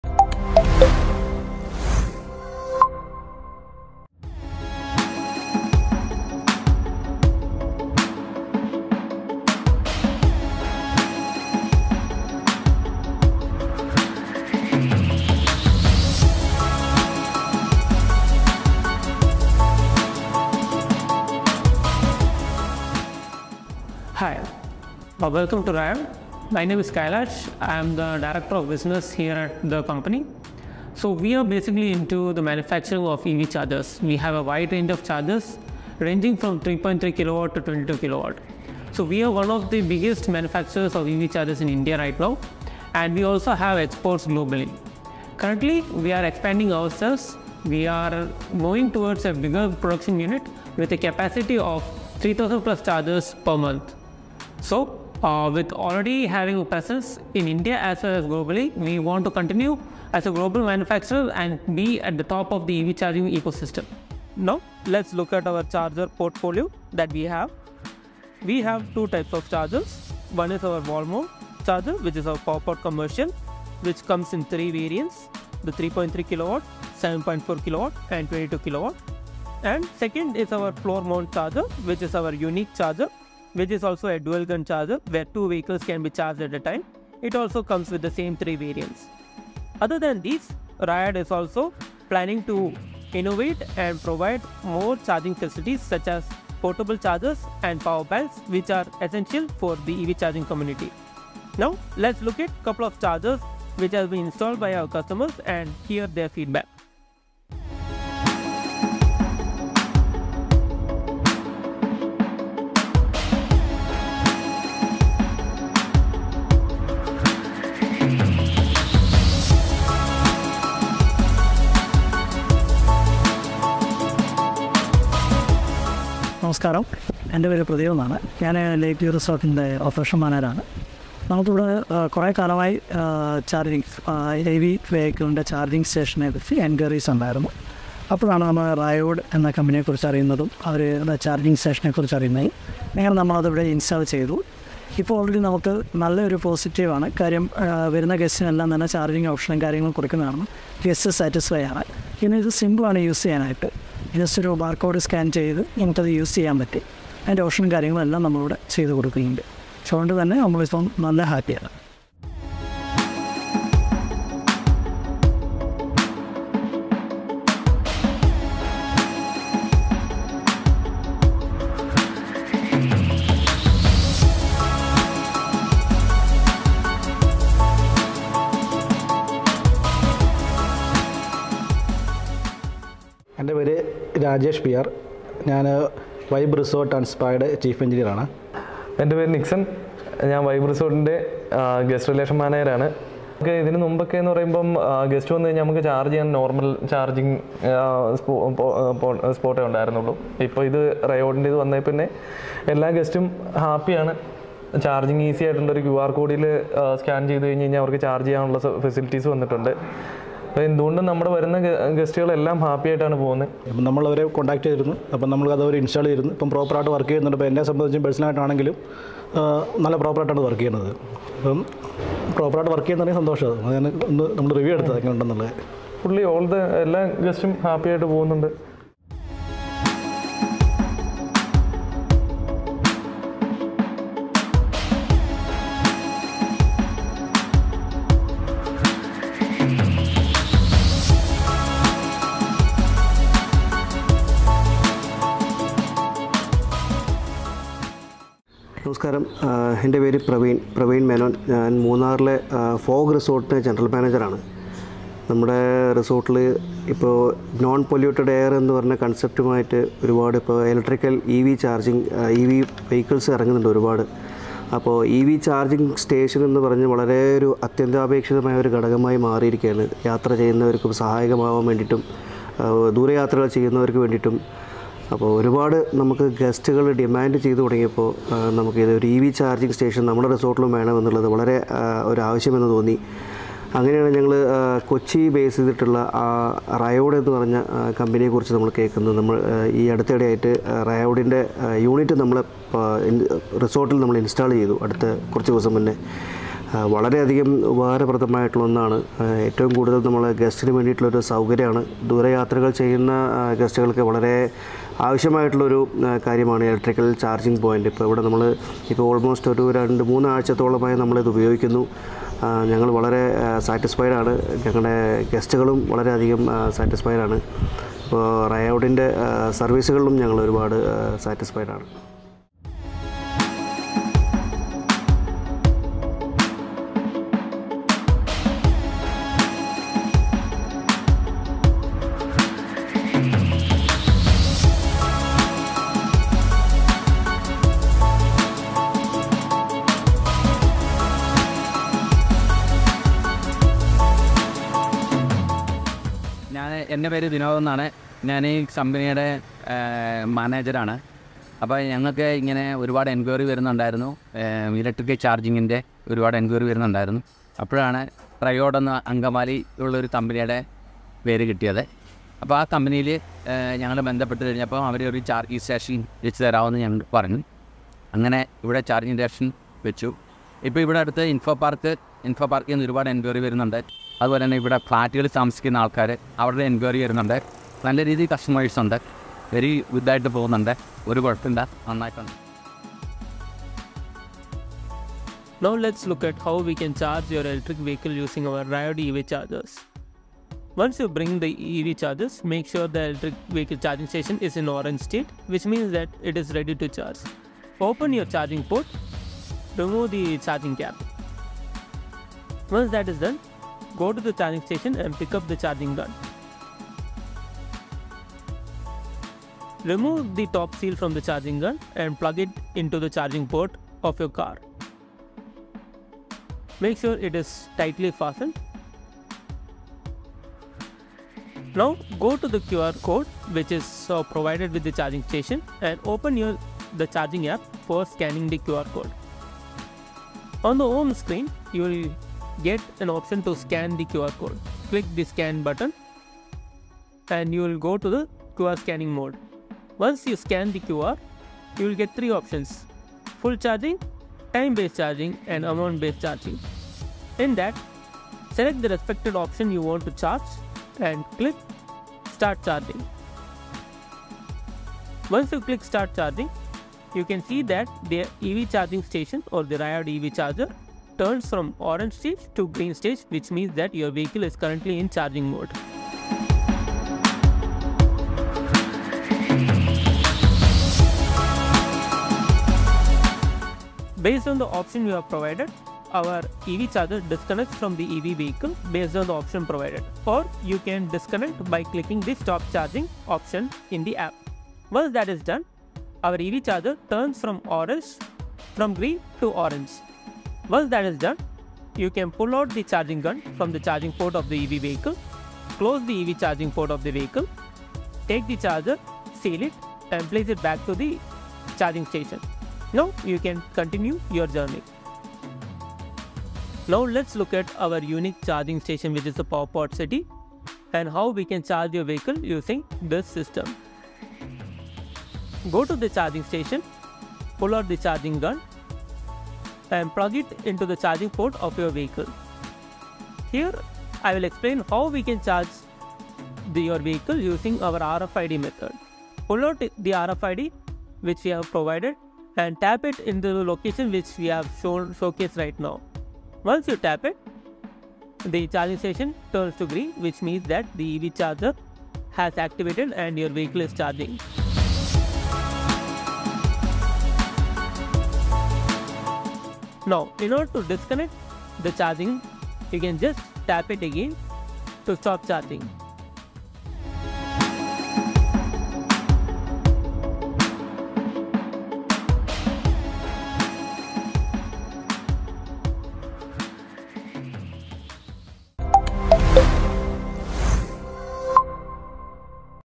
RIOD EV Chargers and Solutions
If you are looking for setting up an EV Charger or charging station to charge your EVs. In this video, we explain the range of EV Chargers that RIOD has under its portfolio. We also have great customer testimonials from where we have already installed our Charging stations.

All the chargers has been designed and developed by us and are made in India. All our chargers are state-of-the-art and have been designed for the Indian conditions.